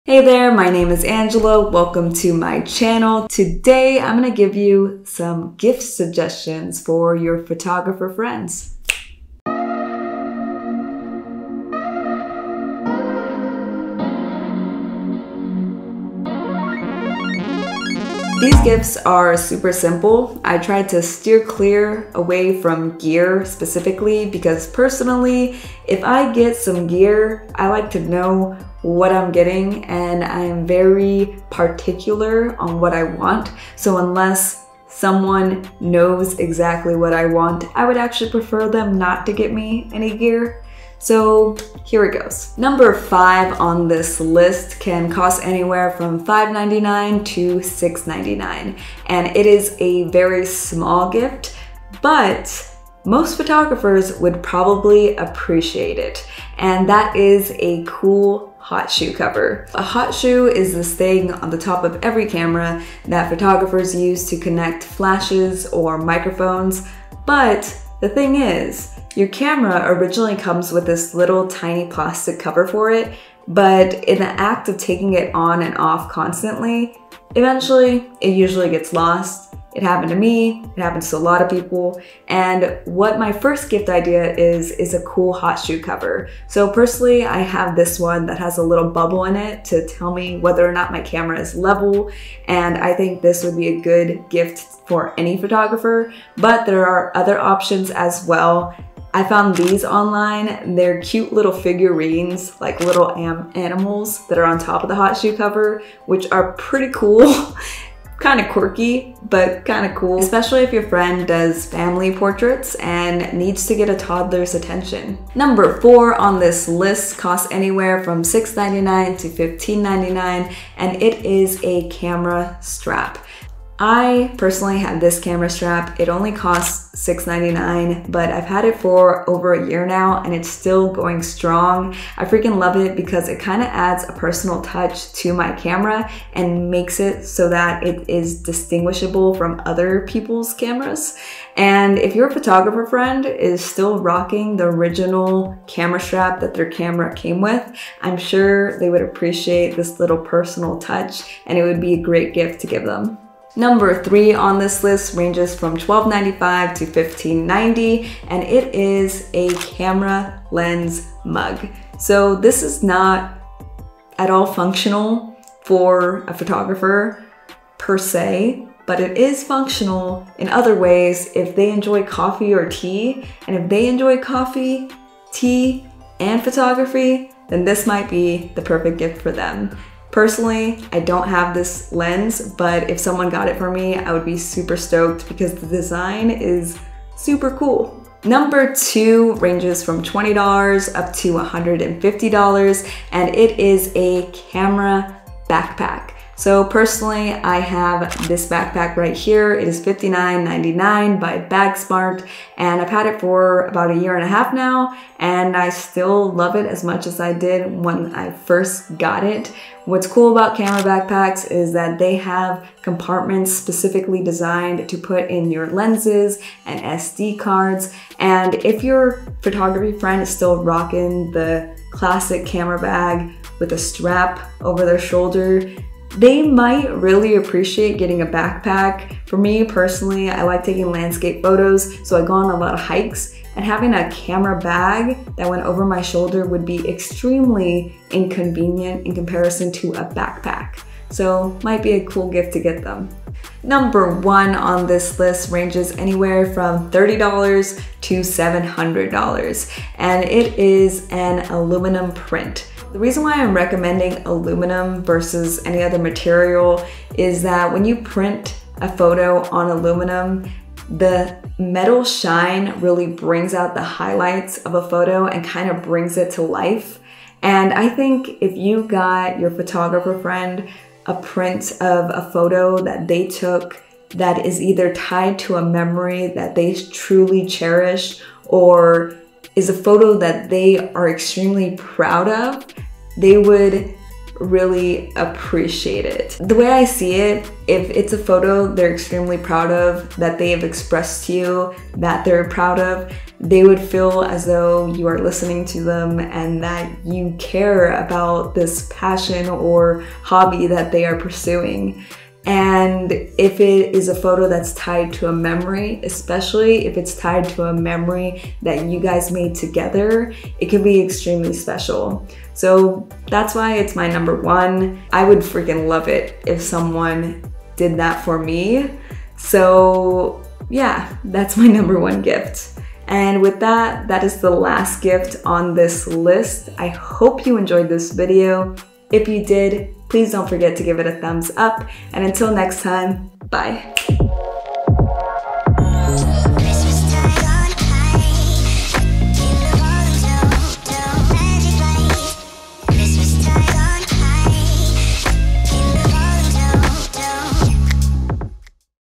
5 Gift Ideas for Photographers [Friday Five]
In this video, I give you 5 gift ideas for the photographers in your life!

Bubble Hot Shoe:

Animal Hot Shoes:

My Camera Strap:

My Backpack:

Camera Lens Mug: